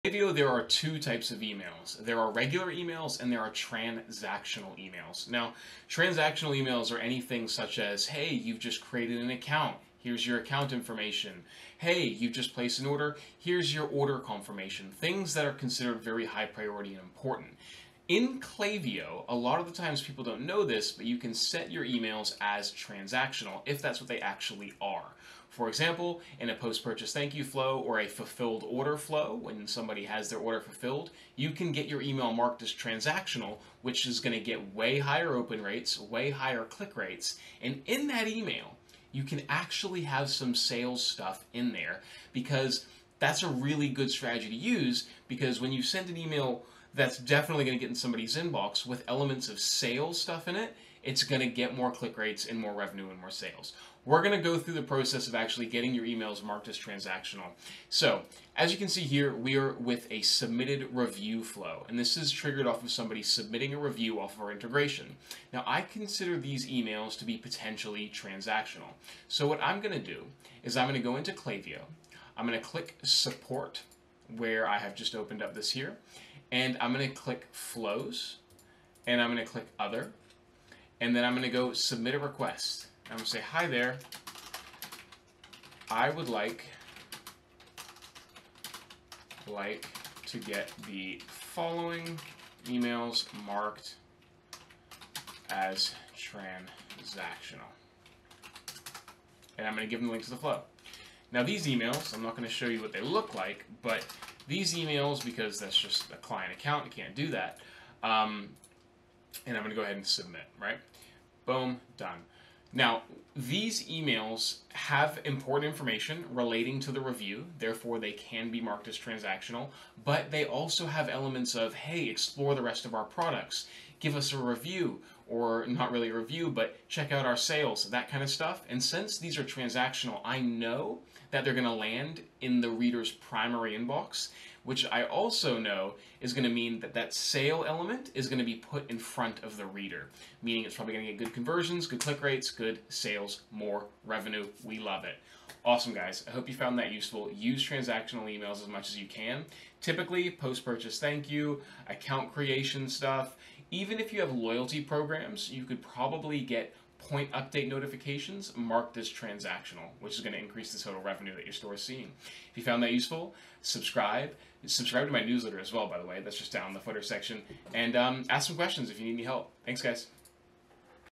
How To Set-Up Klaviyo Transactional Emails
📩 How To Set Up Transactional Emails in Klaviyo (Step-by-Step Guide)

In this video, you'll learn how to mark your Klaviyo emails as transactional — a simple but powerful optimization that can drastically increase your open rates, click rates, and revenue.

Transactional emails are critical for eCommerce: they include order confirmations, account creation alerts, shipping updates, and more. When marked properly, they bypass promotional tabs and land directly in the primary inbox 🔥

Would you like a free audit of your email strategy?

✅ What You’ll Learn:
- What transactional emails are (and how they differ from regular emails)
- When to use transactional emails in Klaviyo (examples: post-purchase, account creation, reviews, loyalty programs)
- How to request Klaviyo to mark emails as transactional
- Why transactional emails can include sales elements (and how this increases conversions!)
- Best practices for eCommerce brands to maximize revenue through transactional messaging

💡 Why This Matters:
Most stores aren’t leveraging this correctly. If you send crucial post-purchase or review emails as “regular,” they may land in spam or get lost. Set them as transactional and ensure your most important messages are seen, read, and acted on.

📬 Tip: Even emails with product recommendations or promotions can qualify as transactional if tied to an account action (like a review submission or point balance update).

🧰 Tools Used:
- Klaviyo (Flow Builder + Support Panel)
- Your ESP’s DNS & Domain Settings (for optimal deliverability)